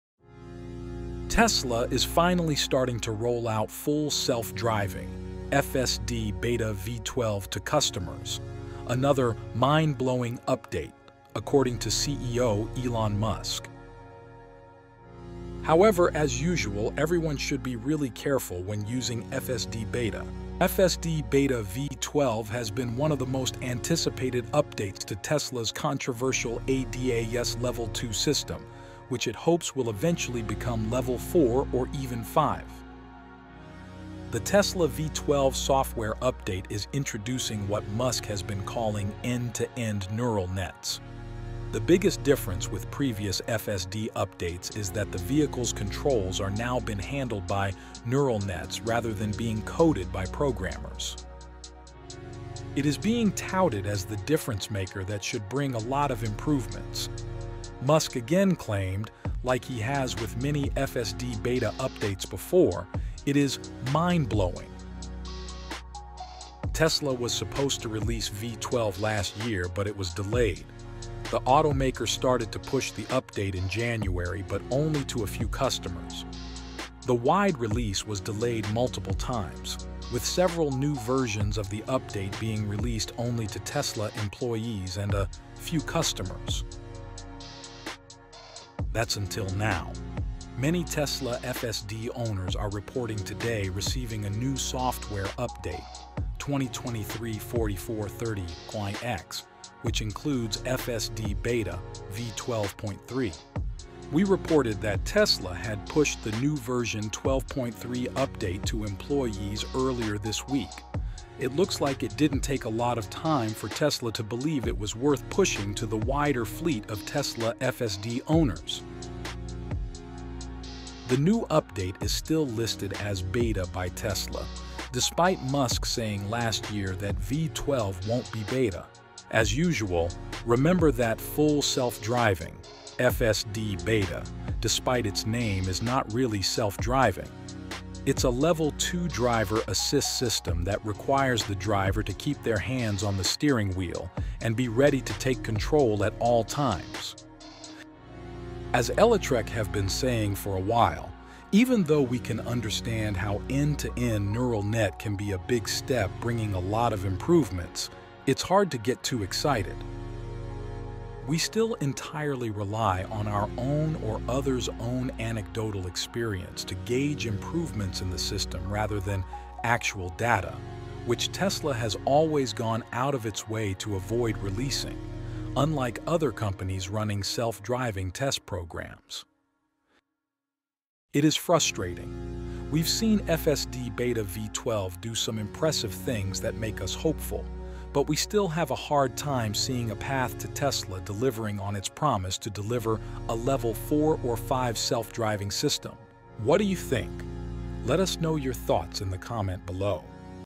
Tesla Full Self Driving Beta v12 finally rolls out – get your minds blown
Tesla is finally starting to roll out Full Self-Driving (FSD) Beta v12 to customers — another “mind-blowing” update, according to CEO Elon Musk.

However, as usual, everyone should be really careful when using FSD Beta.

FSD Beta v12 has been one of the most anticipated updates to Tesla’s controversial ADAS level 2 system, which it hopes will eventually become level 4 or even 5.